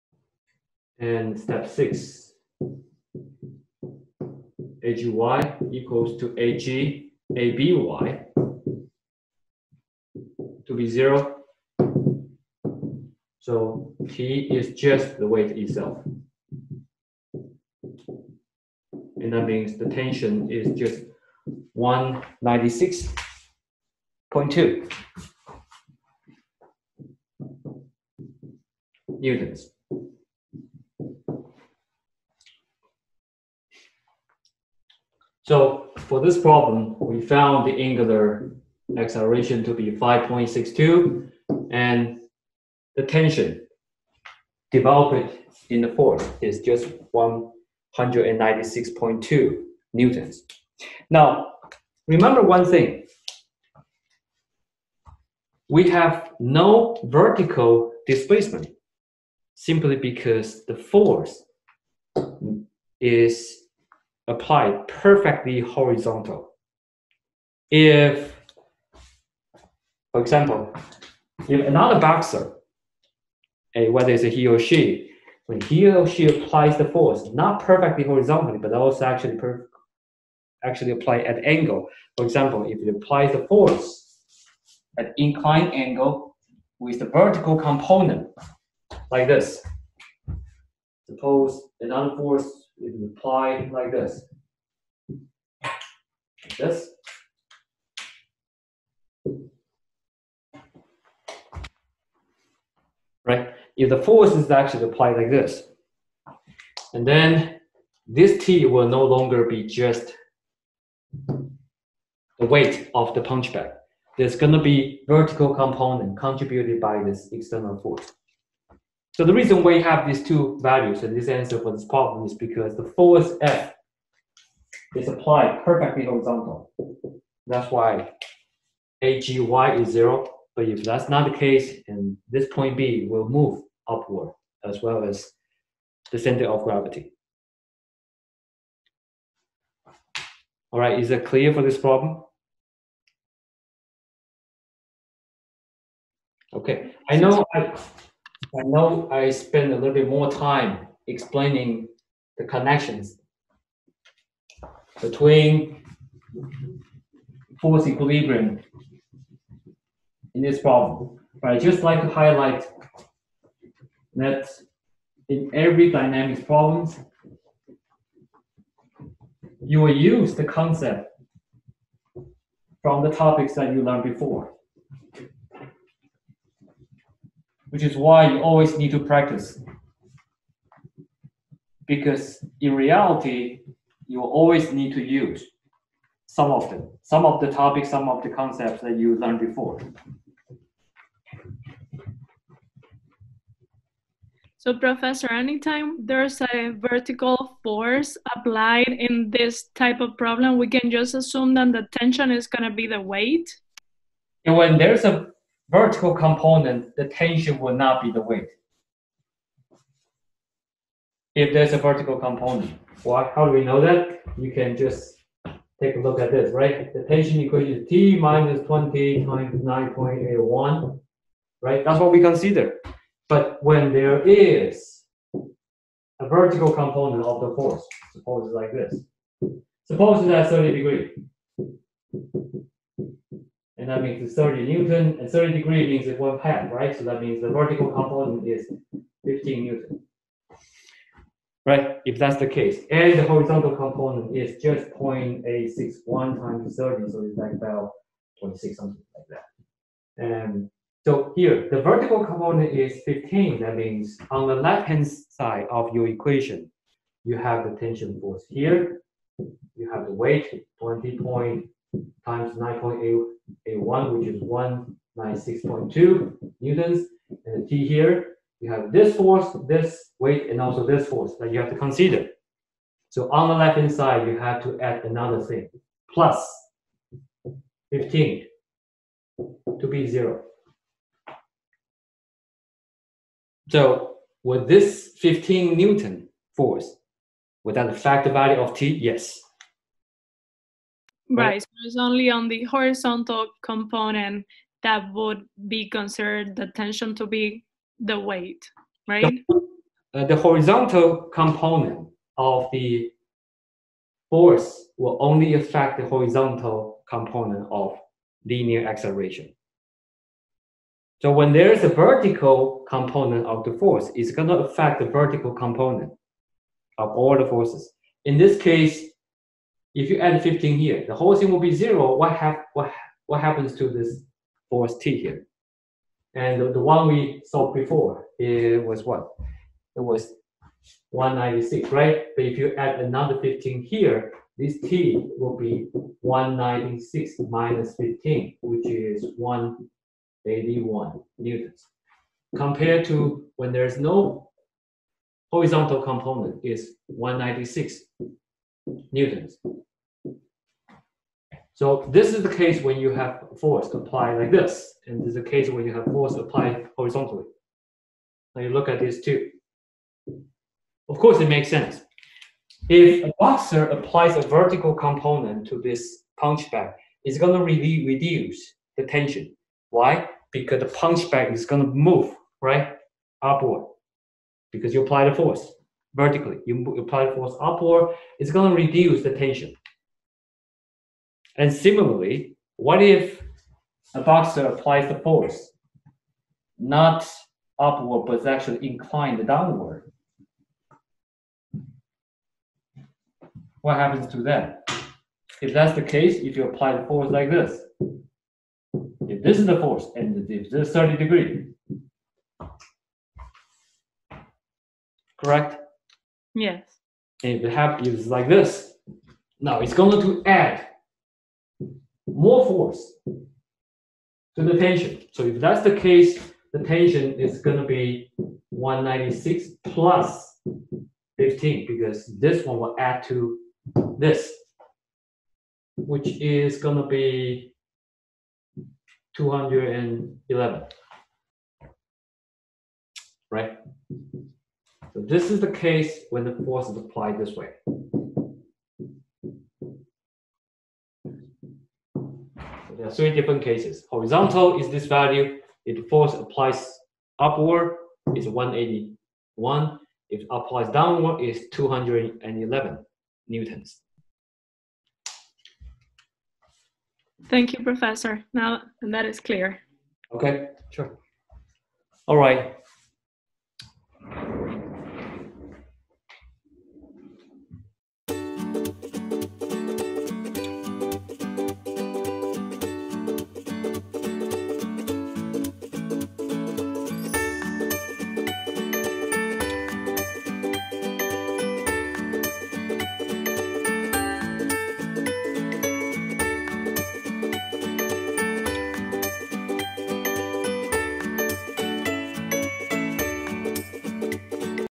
ENGN2070 201 Dynamics__2020 0408__Part 4
This is the online lecture on 04/08/20 for ENGN 2070-201 Dynamics at UMass Lowell, given by Prof. Tzuyang Yu from the Department of Civil and Environmental Engineering. -- Part 4 (last part)
-- Example 28